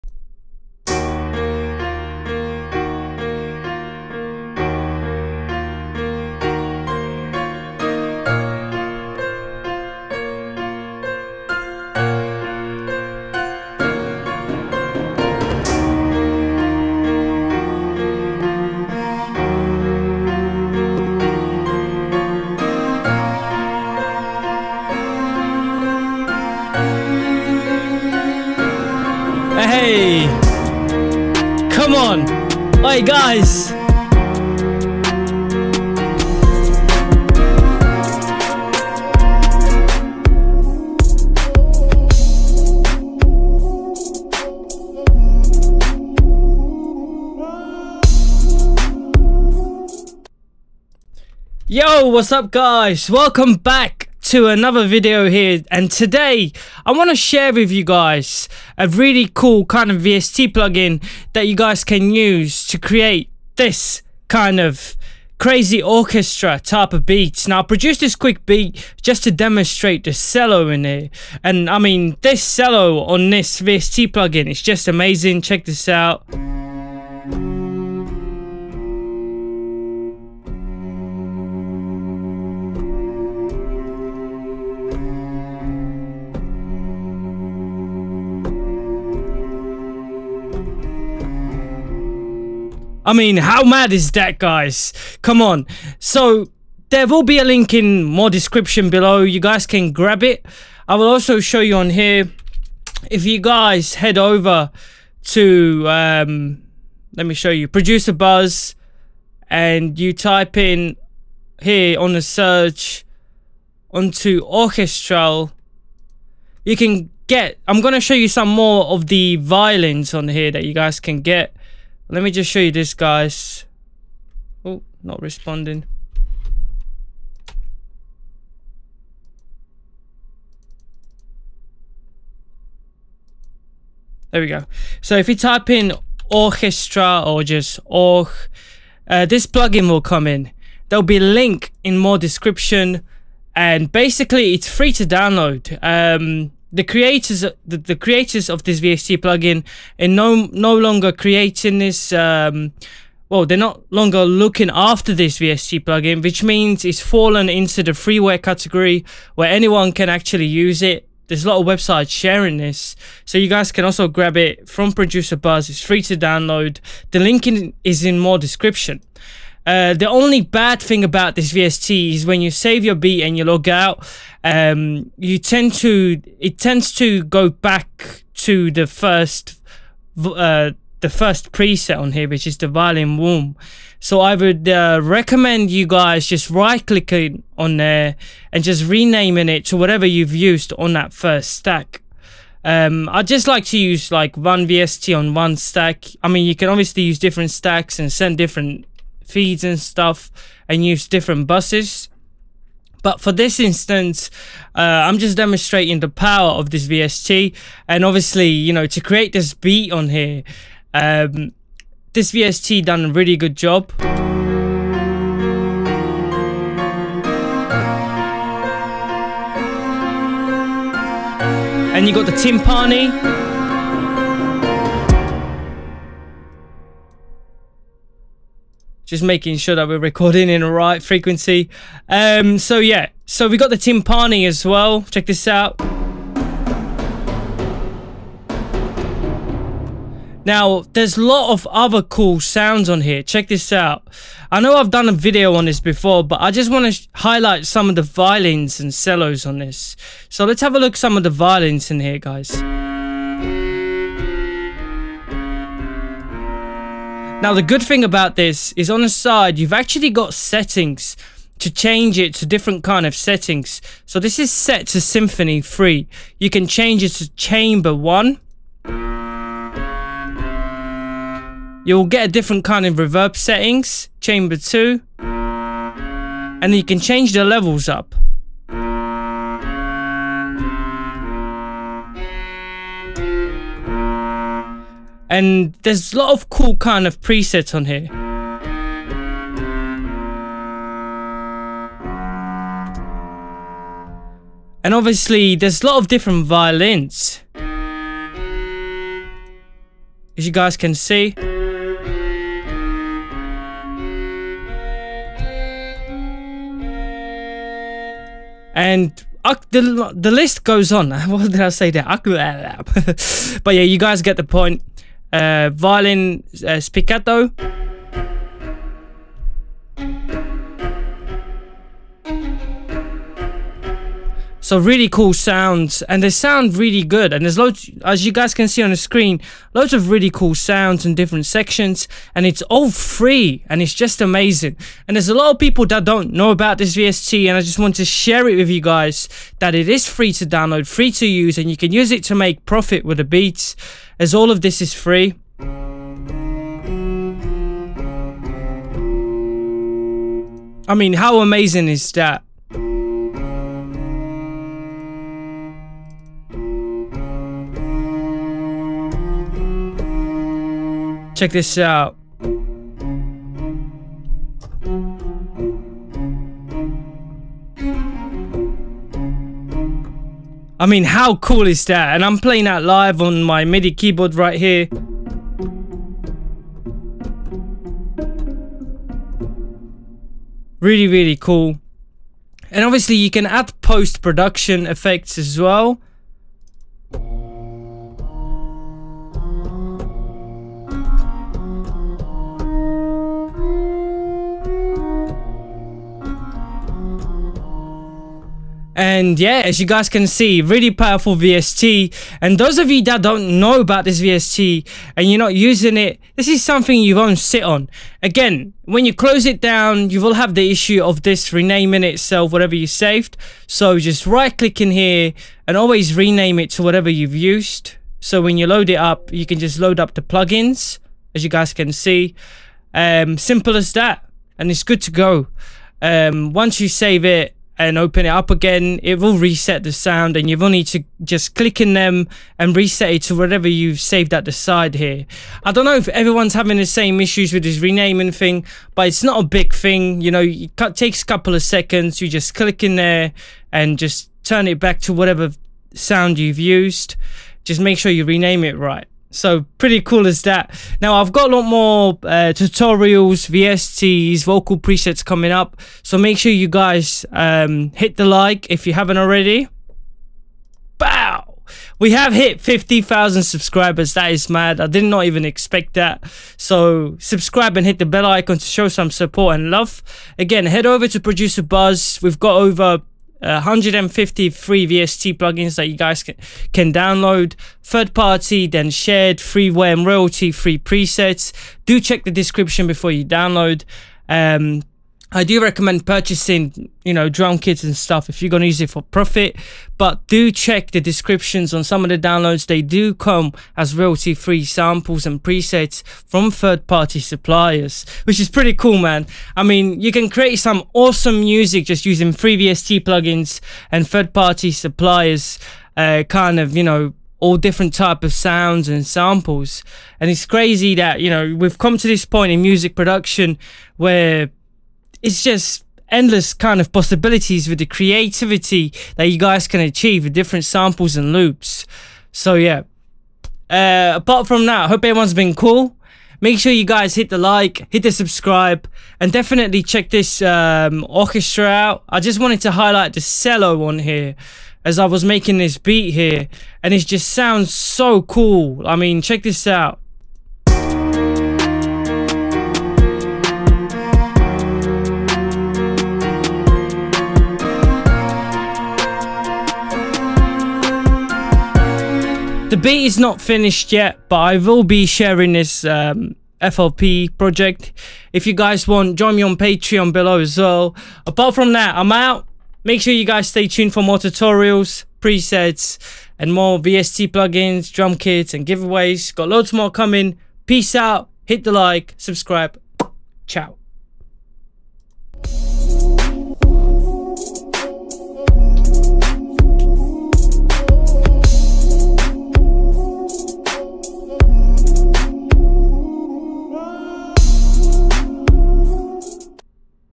Need a Cello or Violin 🎻Free VST Plugin? 🔥 GET THIS 🔥
Need a Cello or Violin? Then get this top free VST plugin!

Hey guys I know that most of you are looking for decent violin or cello VST plugins to make some banging emotional hip-hop and rap beats, so I wanted to share with you this awesome free VST plugin which will definitely do the job. Edirol Orchestral VST is free to download and comes with some amazing free to download violin and cello sounds.

Extra Links to some great free downloads are shown below;

Sub & LIKE to see more! :D thanks